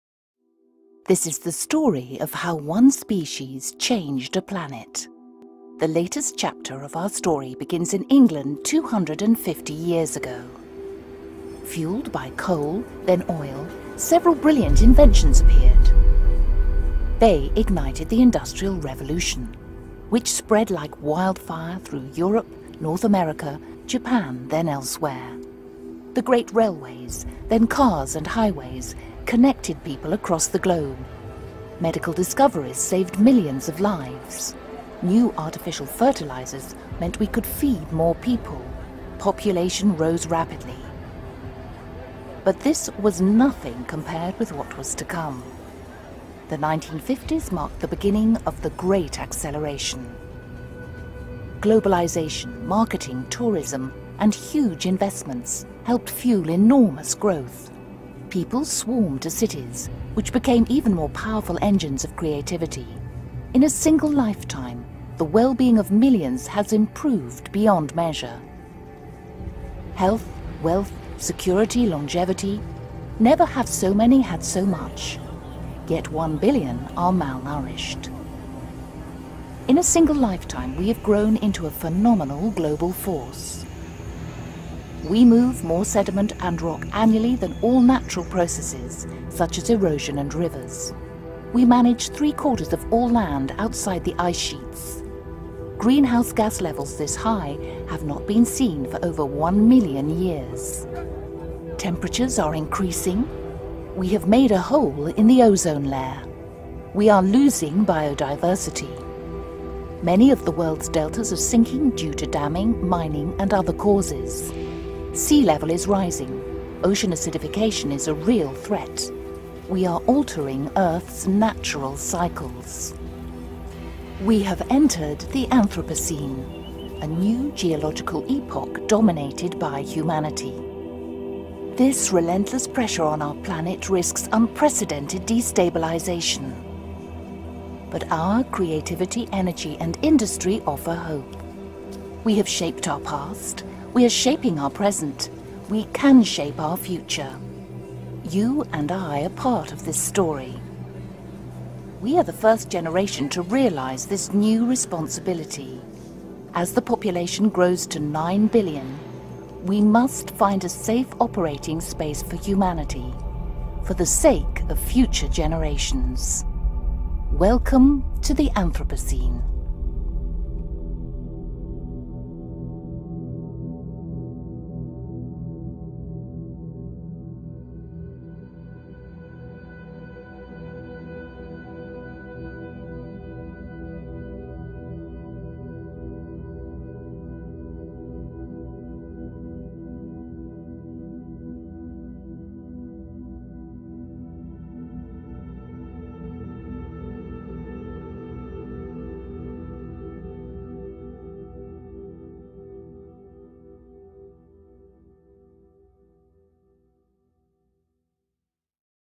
Welcome to the Anthropocene - The Age of Man
A 3-minute journey through the last 250 years of our history, from the start of the Industrial Revolution to the Rio+20 Summit.